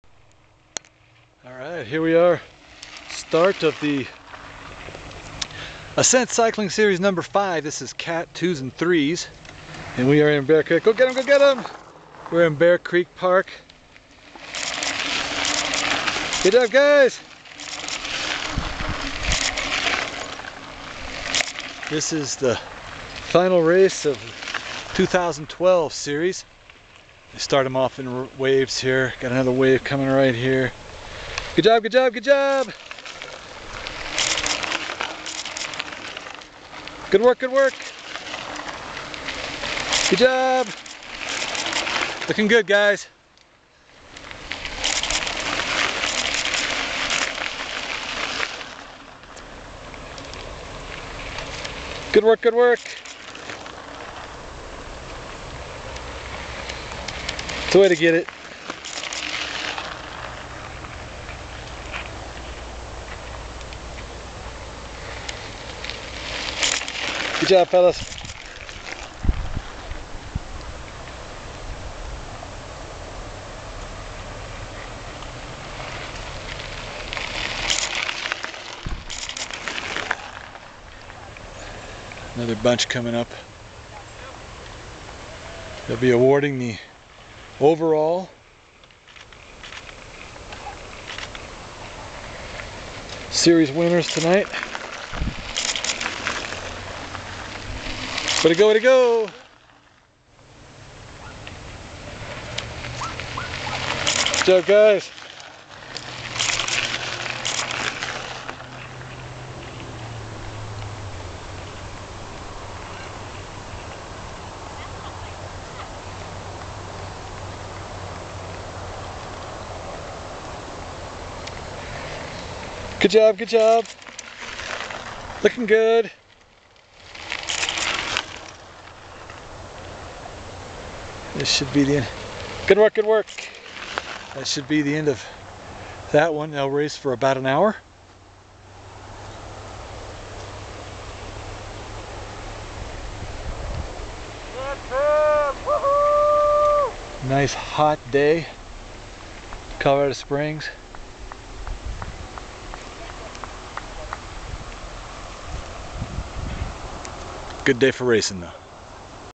Ascent Cycling Series Finale, start of Cat 2 and 3 races
Here's a look at the Cat 2 and 3 racers in the final race of the 2012 Ascent Cycling Series at Bear Creek Park in Colorado Springs.